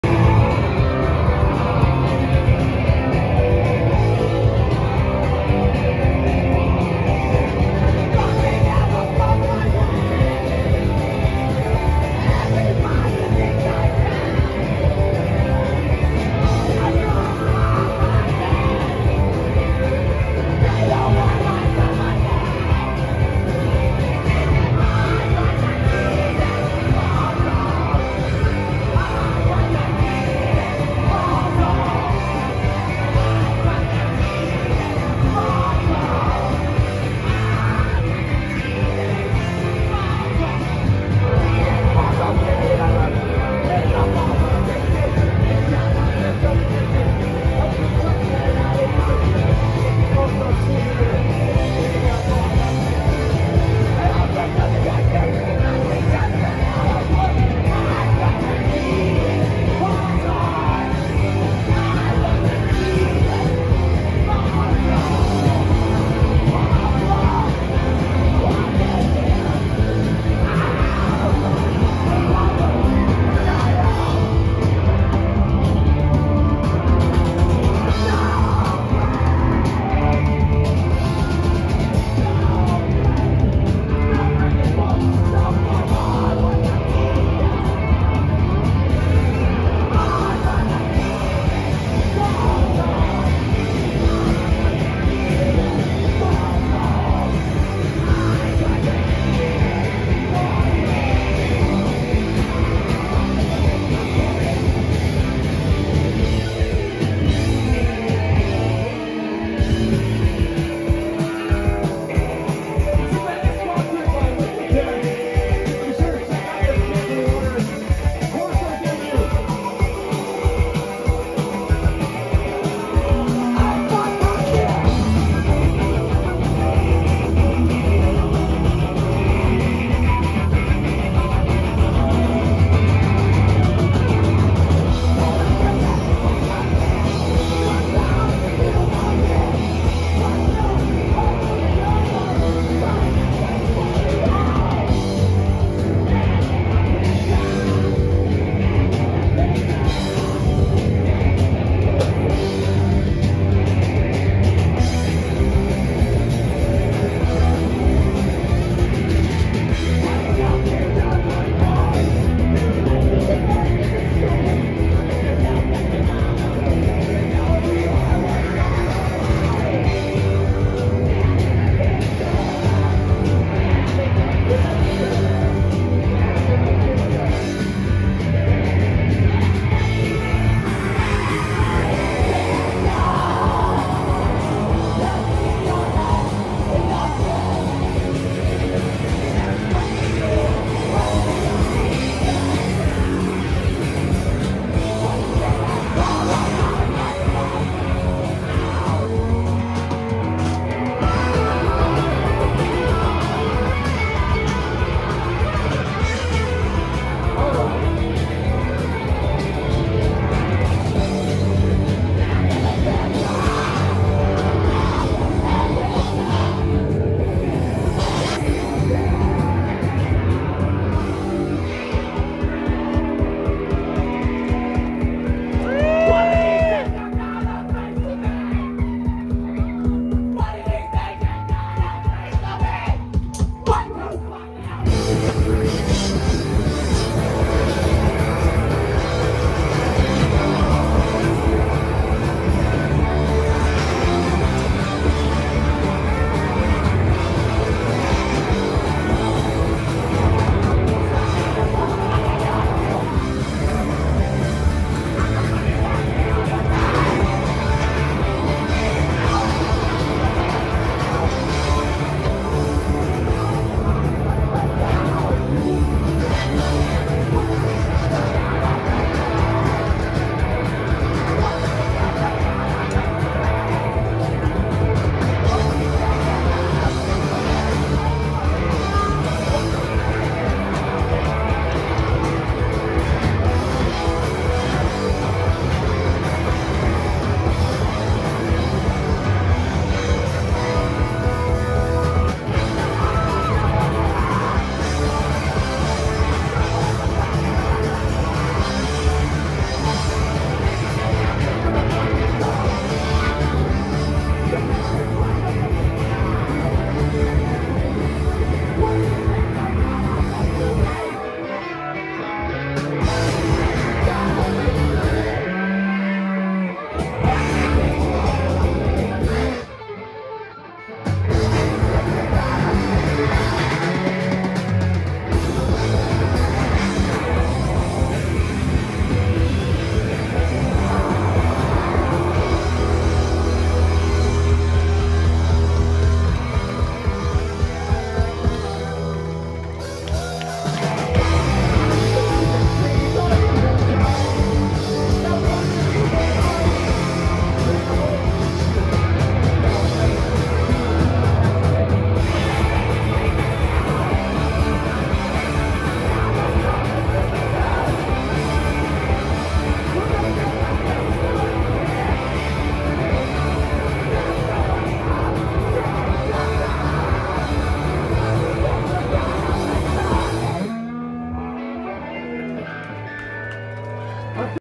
Lumpy & The Dumpers @ TV Eye 9/17/23
Lumpy show breaks out in the middle or maybe end of Alien Nose Job show at TV Eye on 9/17/23 - dotdash